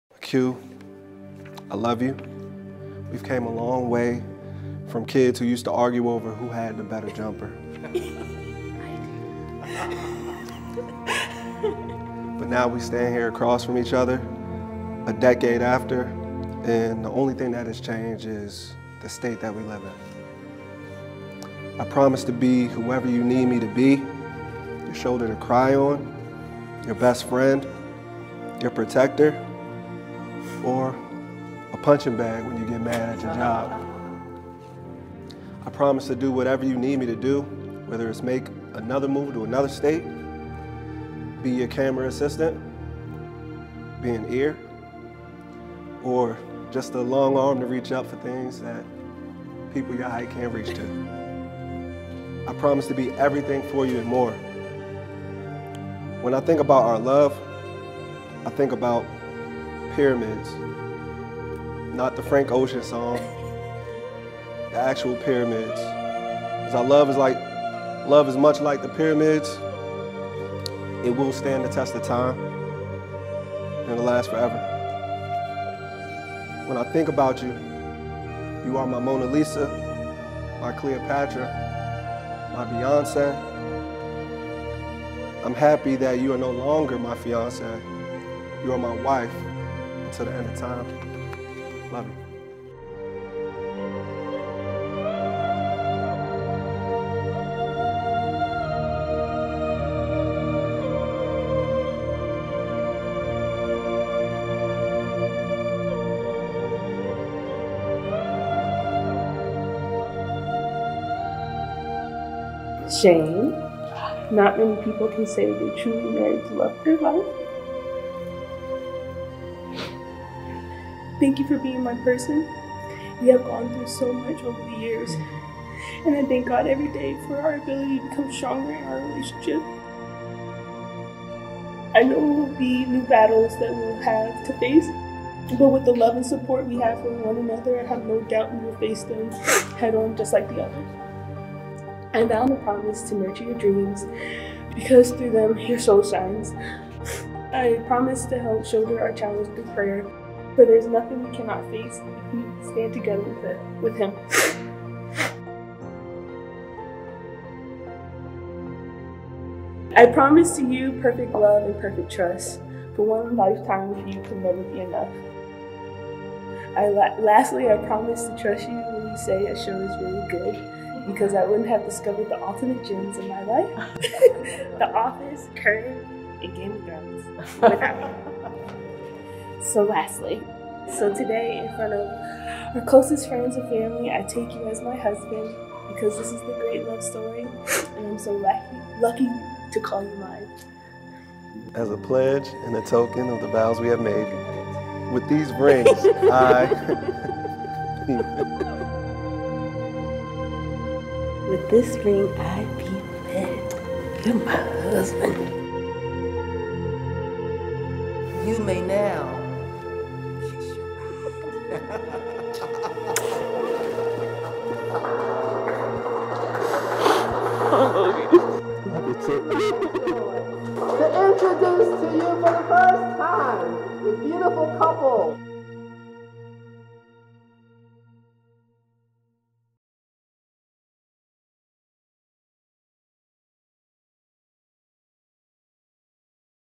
Shane and Quelle at the Main Street Ballroom Ellicott City
On March 13th, 2021, Shane and Quelle were able to get married on their original wedding date. There were times when they thought they would have to postpone the wedding or even uninvite the entire guest list. They weathered the storm of COVID19 and were able to exchange their vows at the Main Street Ballroom in Ellicott City, MD.

Event Planner: Events By Lady J, LLC.
Violinist: Jen Violin Jenviolin
Photographer: Katherine Elizabeth Photography
Officiant: Pastor Ostein Truitt
Make Up: Ronkeya Hannon @sparkle627
Hairstylist: Desi Kelly Tresses unlimited
Floral Designer: Nadine De Leon Nadine De Leon Designs